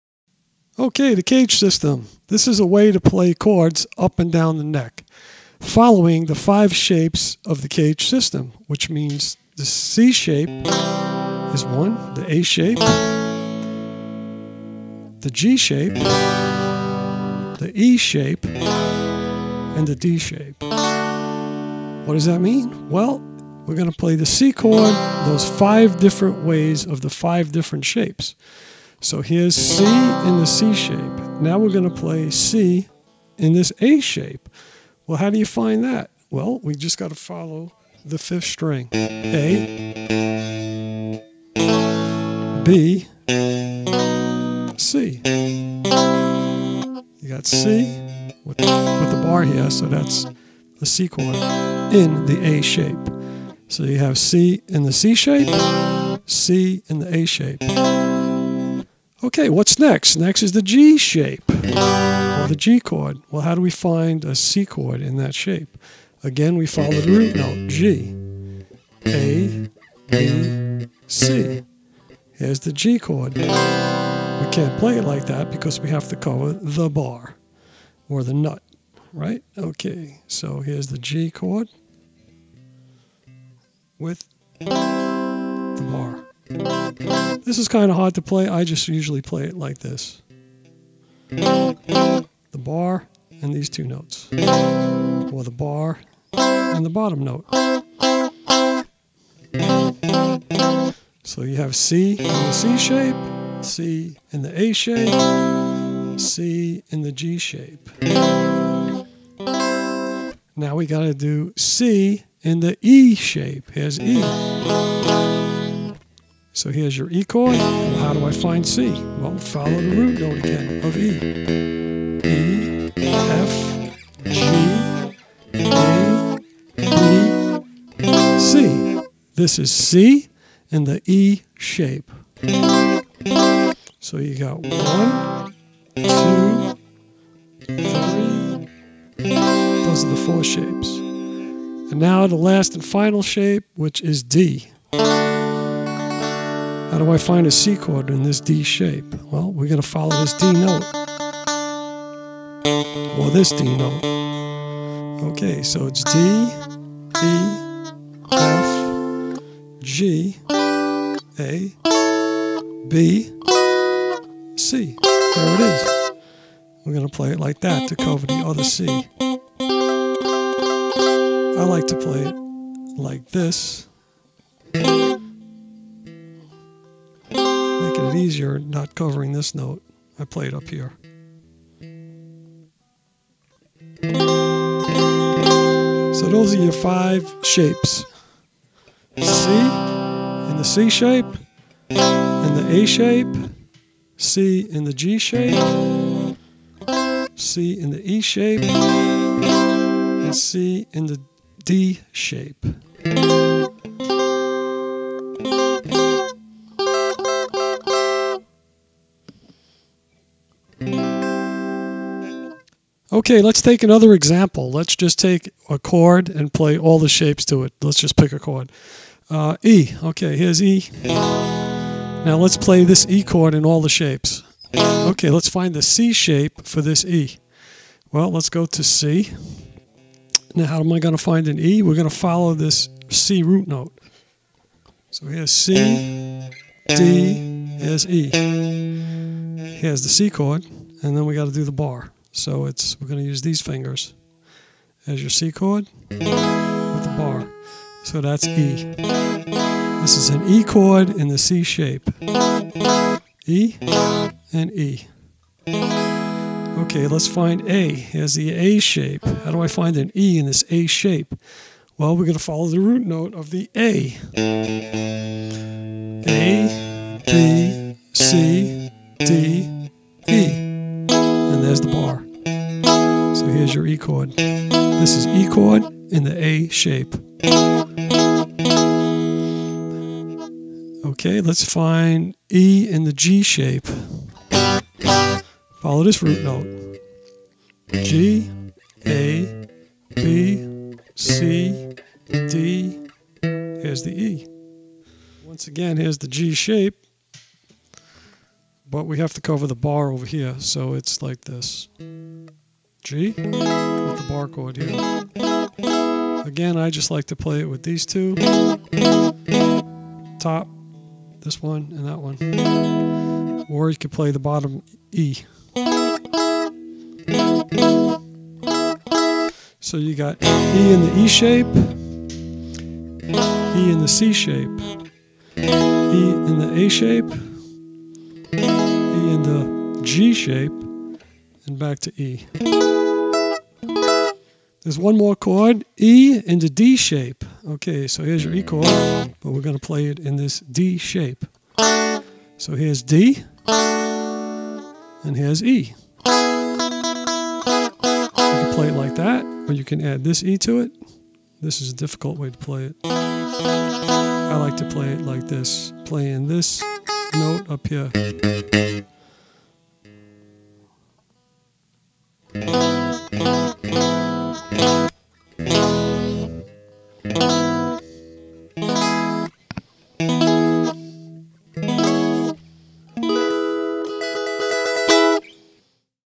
Caged System - Chords - 5 positions - Guitar lesson
Follow the shapes and the root note, of the 5 simple chords C-A-G-E-D.